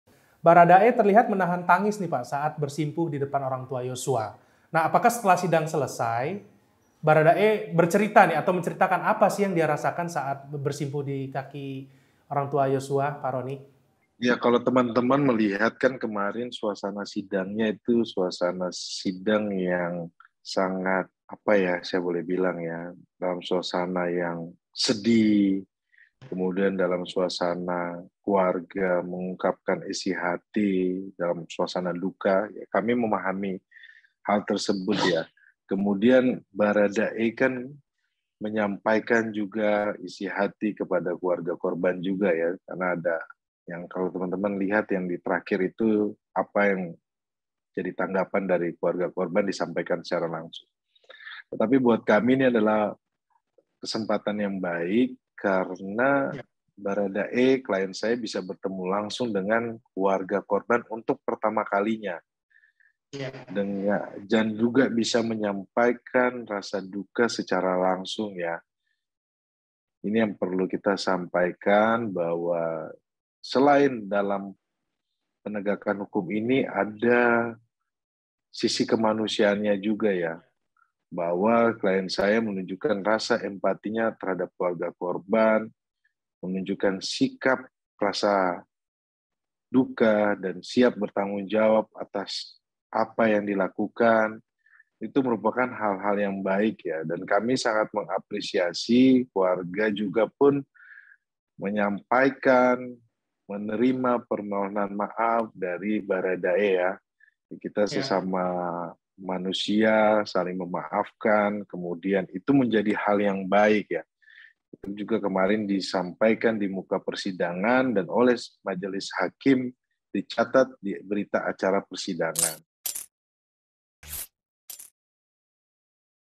BHARADA E BERTEMU ORANG TUA BRIGADIR J, KUASA HUKUM: KESEMPATAN YANG BAIK
Kuasa Hukum Richard Eliezer Pudihang Lumiu atau Bharada E, Ronny Talapessy mengatakan pertempuan kliennya tersebut dengan orang tua Brigadir Nofriansyah Yosua Hutabarat atau Brigadir J merupakan kesempatan yang baik. Sebab, Bharada E bisa bertemu langsung dengan orang tua Yosua guna menyampaikan permintaan maaf secara langsung atas perbuatannya.

Menurut Ronny, selain dari penegakan hukum, kliennya bisa menunjukan rasa empati, sikap rasa duka dan siap bertanggung jawab atas perbuatannya. Selain itu, Ronny meyakini bahwa hal baik tersebut dicatat oleh majelis hakim.